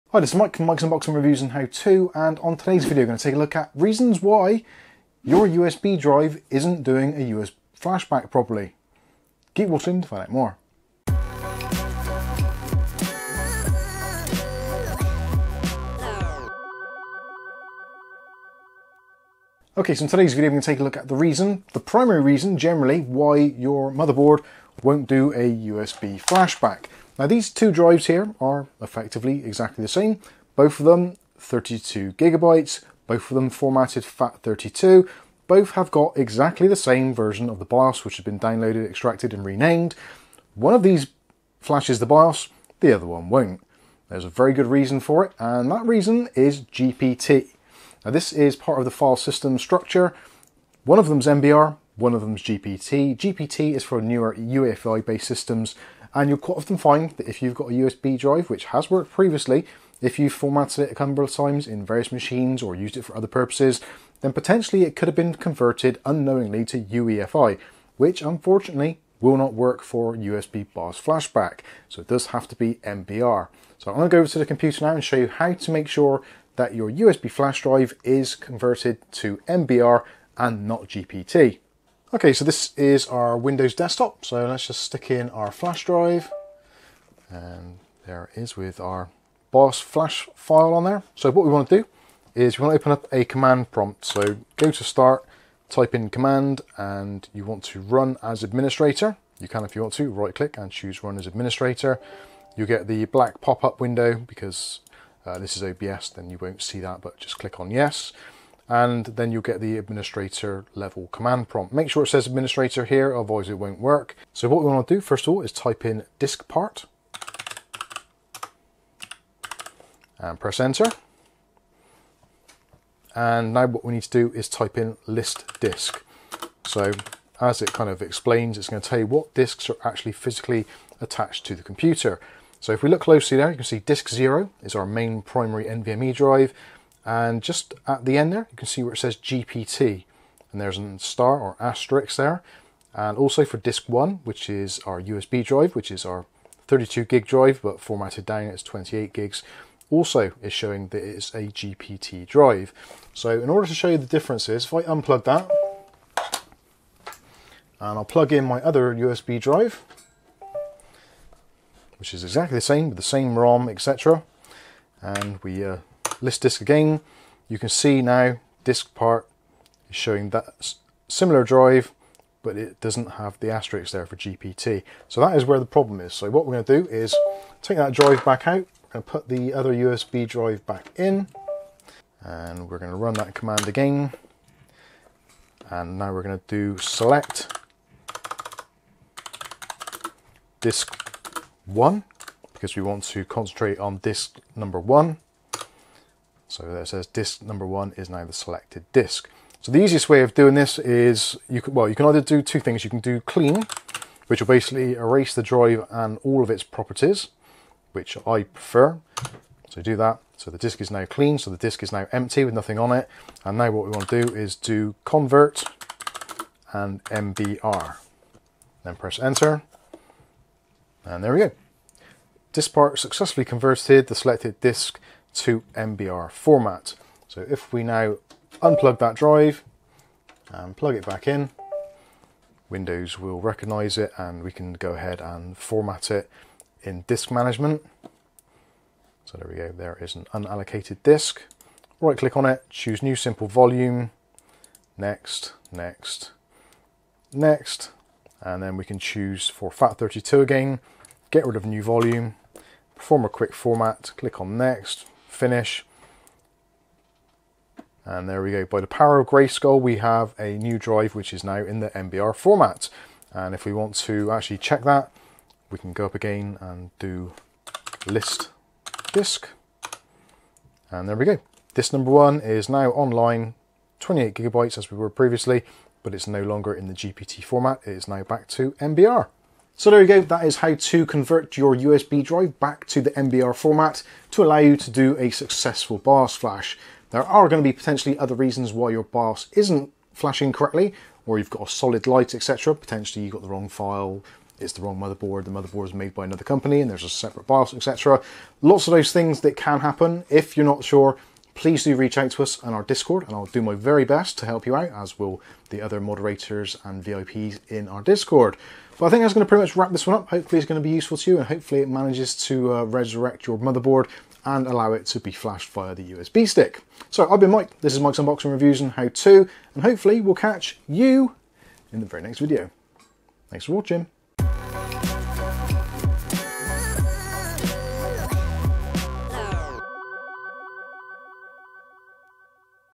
USB BIOS Flashback Solid LED Not Flashing Most Common Reason Convert To mbr MBR
USB BIOS flashback solid LED not flashing most common reason
convert your drive to MBR from GPT
RECOMMENDED USB DRIVE
Amazon Local
Ebay UK
Ebay US
Ebay CA
Ebay AUS

When your BIOS refuses to FLASH this is the number one reason that it fails if the BIOS file is correct and in the right format.
Sometimes the USB stick will be converted to GPT rather than the more compatible MBR format.
So how can we switch the GPT back to MBR?

Pretty easy with some CMD prompt commands.
Use the DISKPART command
and the CLEAN option
and also the CONVERT MBR commands to restore your drive quickly as easily